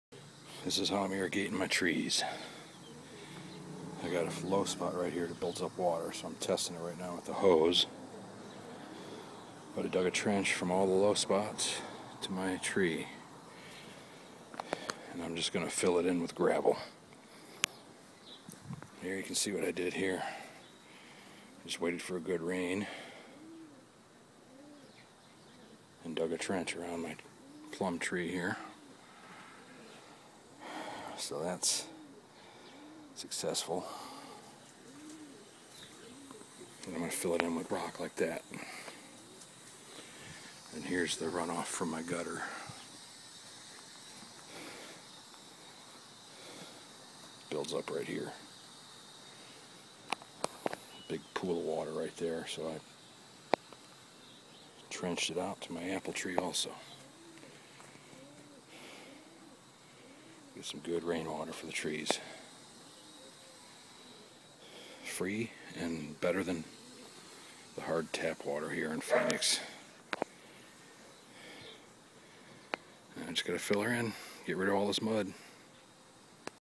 Small scale earthworks(rain water diversion to fruit trees urban style)
I had a wild hair after some heavy rains pooled up in a couple low spots in my yard. Now my trees will actually get a good rain water soaking during the storms. I just used a trench shovel and dug a little river leading to the trees and I didn't even need surveyor's equipment, lol. Time will tell. Some permaculture people call them swales. These are a variation of that.